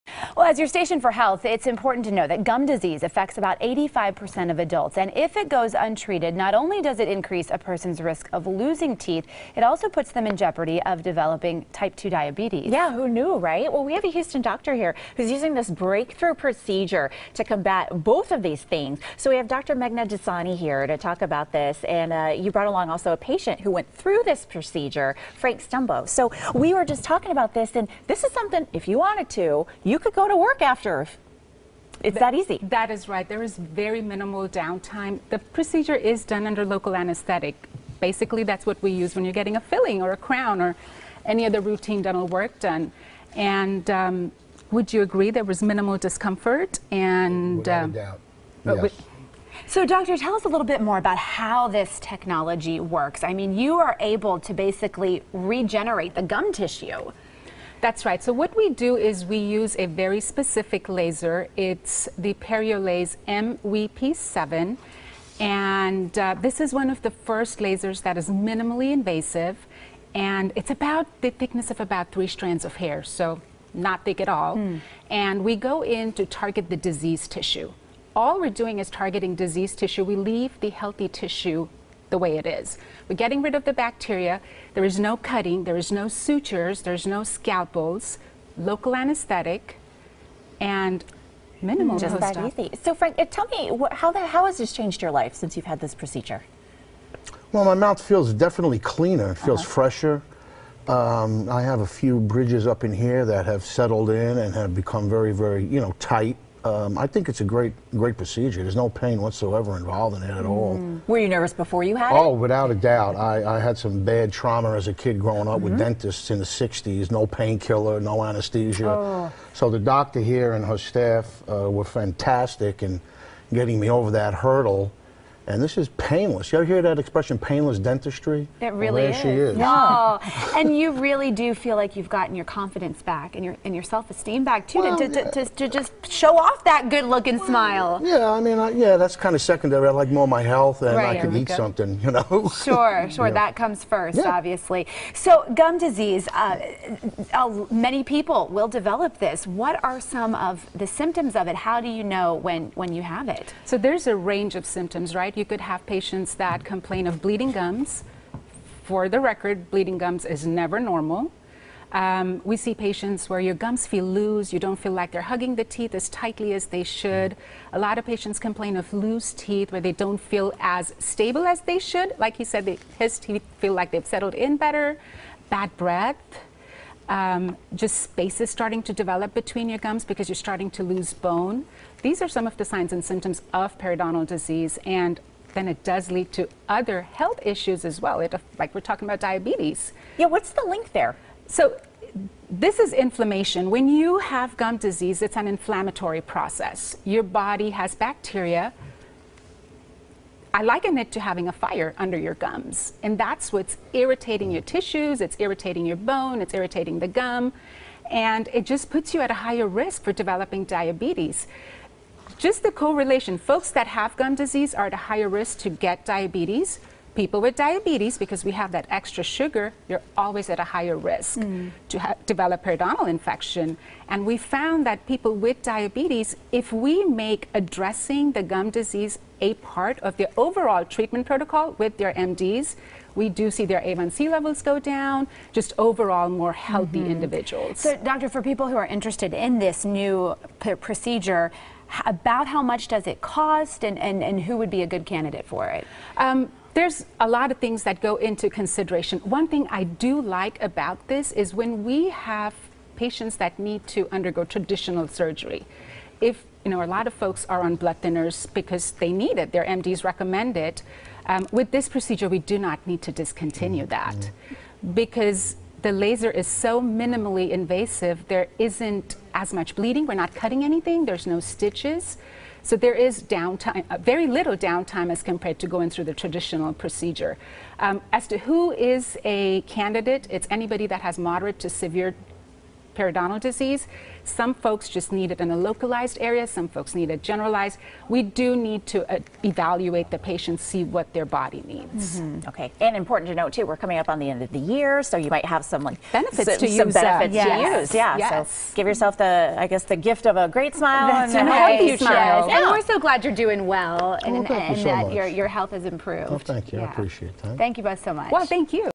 Houston's Dr. Dassani on How LANAP Treatment Combats Gum Disease & Type 2 Diabetes
Dr. Dassani became LANAP trained in part because of the systemic connection between gum disease and several other serious illnesses, including type 2 diabetes. Appearing on Fox 26 Houston, she discusses how LANAP treatment allows her to help her patients' total health.